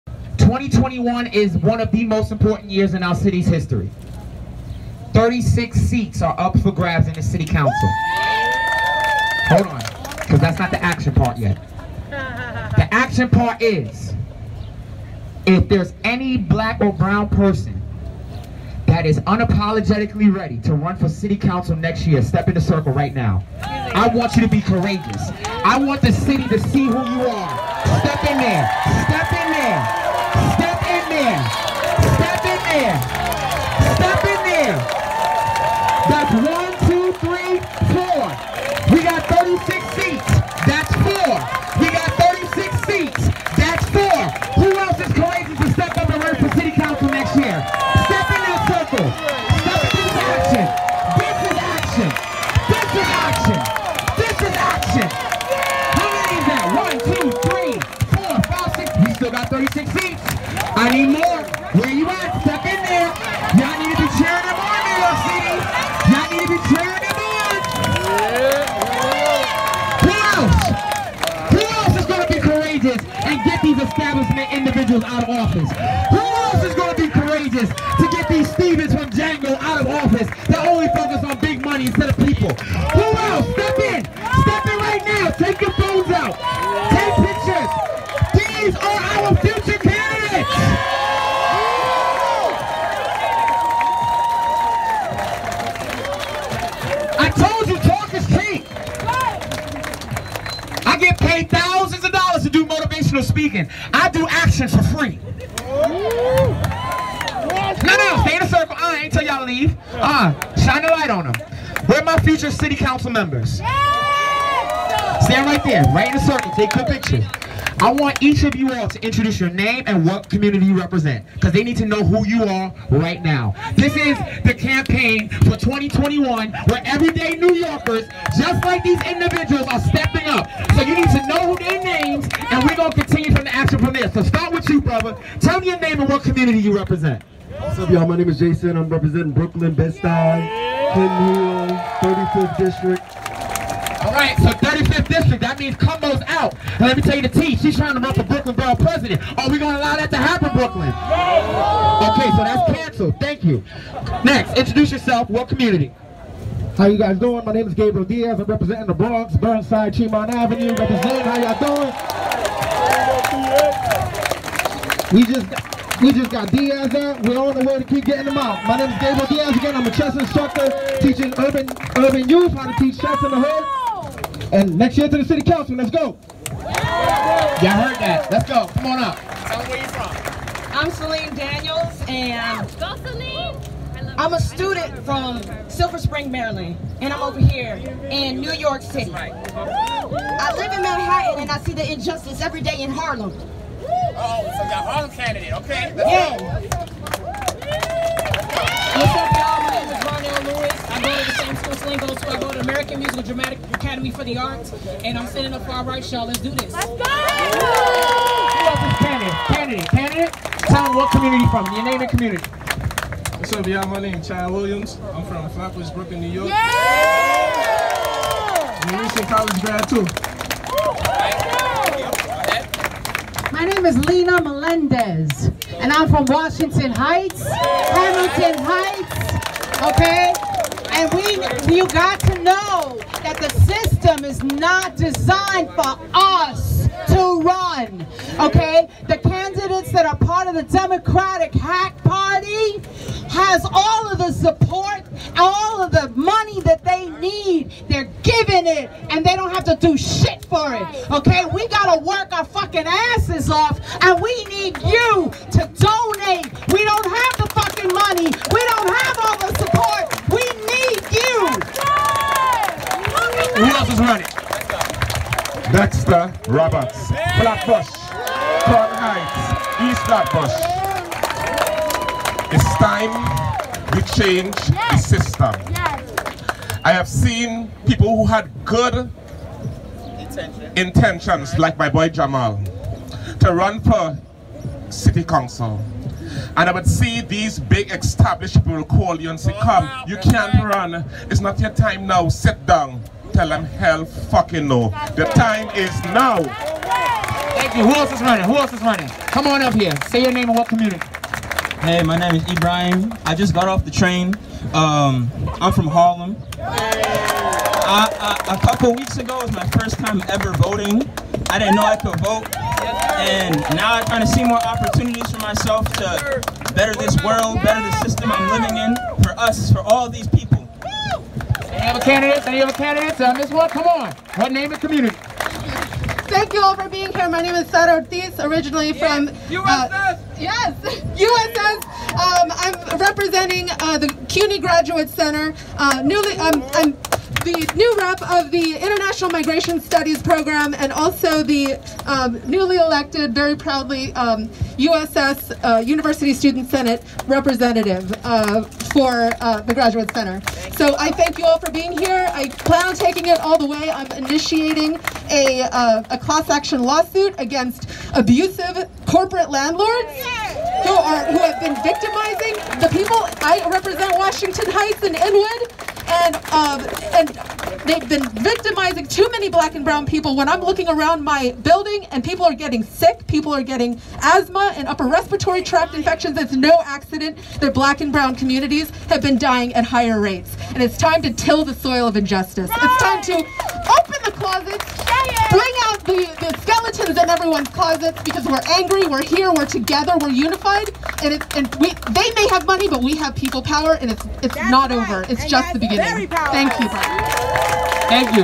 Occupy City Hall Brings the New City Council
The protesters at City Hall bring their City Council candidates for 2121 to the circle. 36 open seats for City Council, and the Occupy City Hall protesters bring it on